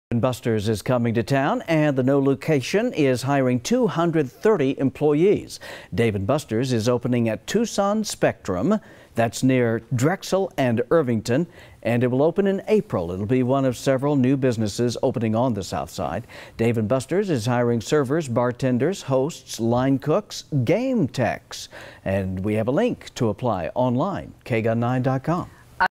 Dave & Busters to hire 230 for Tucson location
Dave &  Busters is expanding to Tucson's southside and will hire 230 workers.